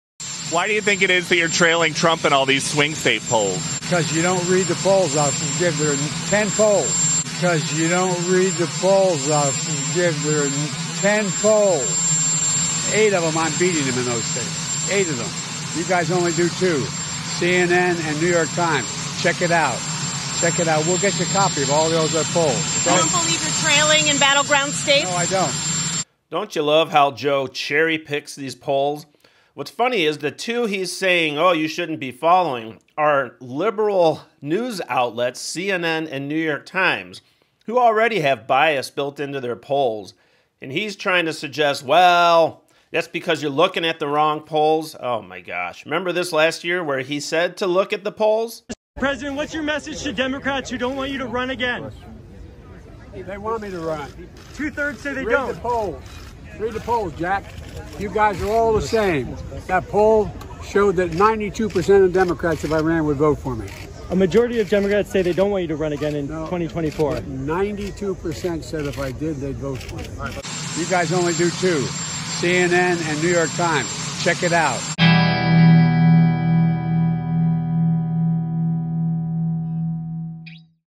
Biden tells Doocy he’s looking at the WRONG POLLS…
Q: Why are you trailing in swing states?

BIDEN: "Because you don't read the polls out!"

Q: "You don't believe you're trailing in battleground states...?"

BIDEN: "No"